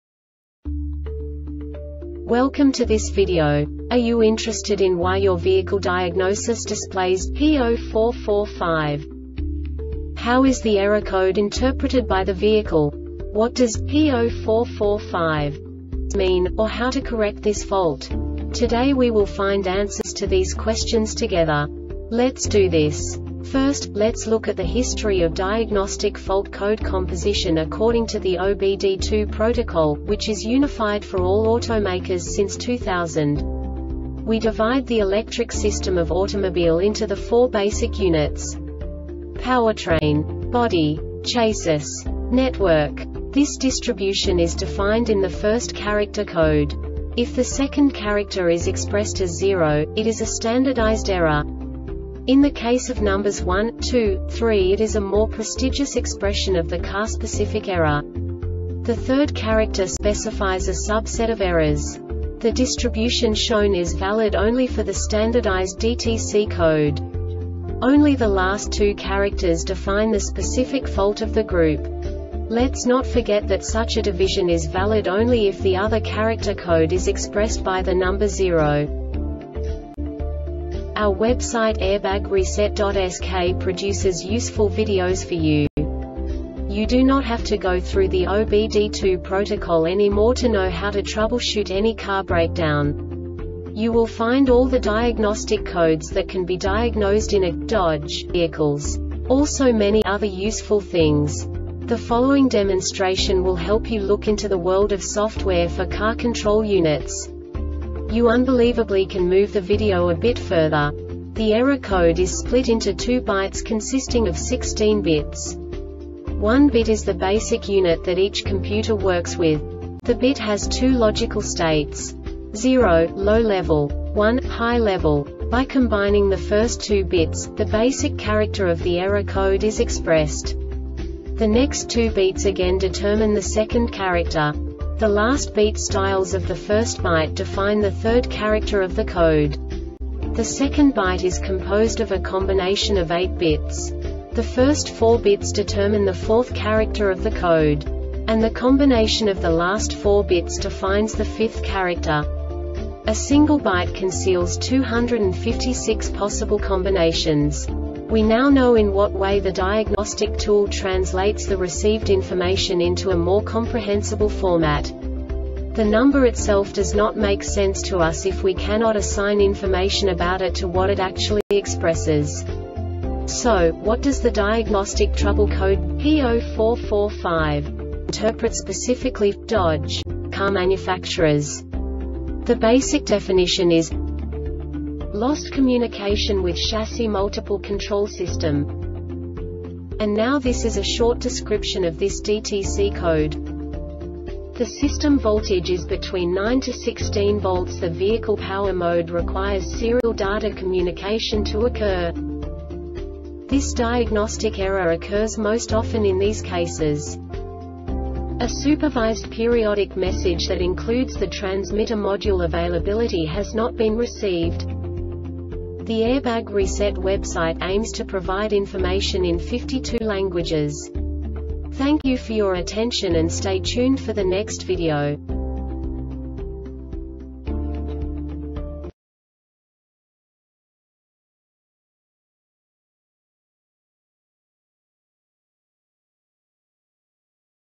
DTC Dodge P0445 Short Explanation
Contents:
0:21 Basic DTC analysis according to OBD2 protocol standard.
1:48 Insight into programming
2:58 A diagnostically understandable description of the Diagnostic Trouble Code
3:05 Basic error definition

Make:
Dodge
Code:
P0445
Definition:
Lost Communication With Chassis Multiple Control System
Description: